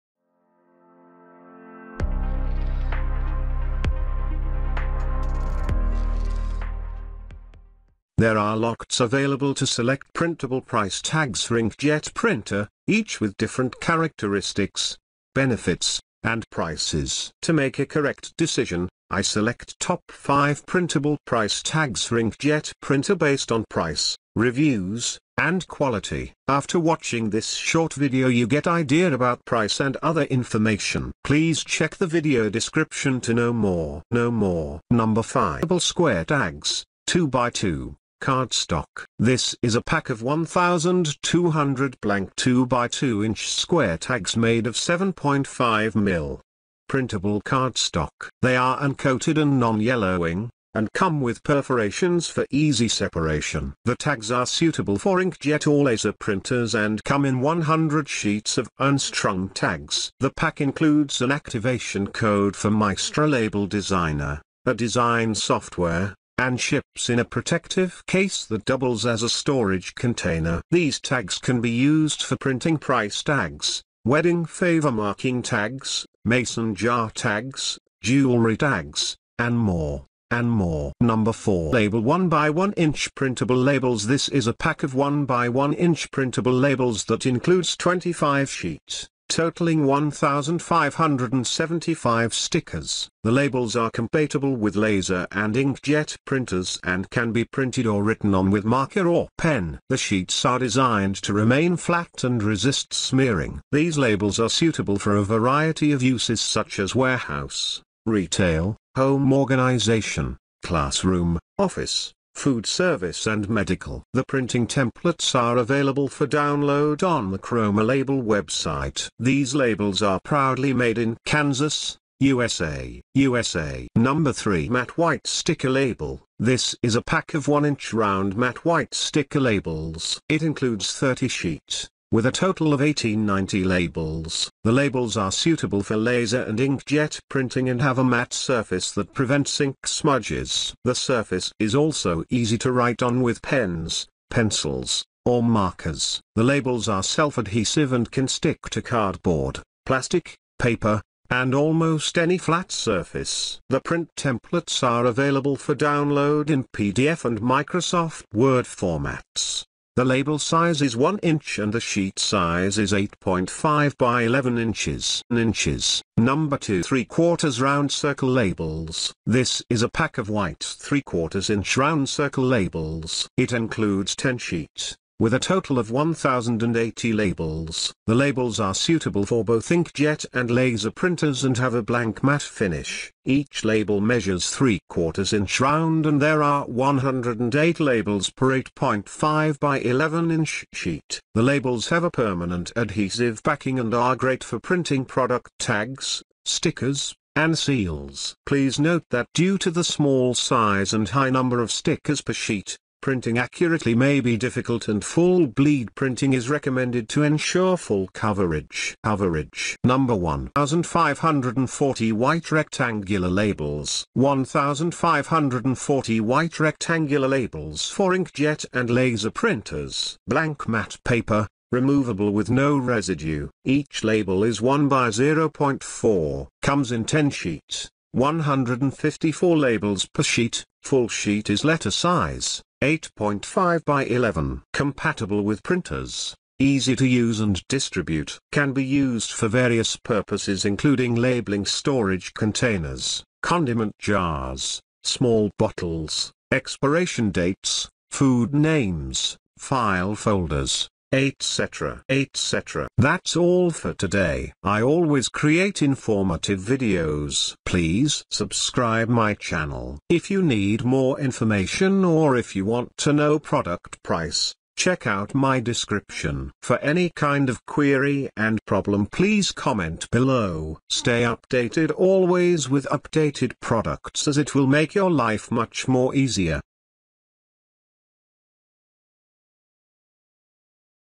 Top 5 Printable Price Tags For Inkjet Printer || Printer Expert
In this video, we will show Top 5 Printable Price Tags For Inkjet Printer
✅ Here Are The List List Of Top 5 Printable Price Tags For Inkjet Printer
Number 5 : Printable Square Tags - 2 x 2 - Cardstock
Number 4 : ChromaLabel 1 x 1 Inch Printable Labels
Number 3 : 1" Round Matte White Sticker Label
Number 2 : White 3/4" Round Circle Labels
Number 1 : 1540 White Rectangular Labels

0:00 - Intro
0:35 - Printable Square Tags - 2 x 2 - Cardstock
1:21 - ChromaLabel 1 x 1 Inch Printable Labels
2:04 - 1" Round Matte White Sticker Label
2:46 - White 3/4" Round Circle Labels
3:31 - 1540 White Rectangular Labels
4:12 - Conclusion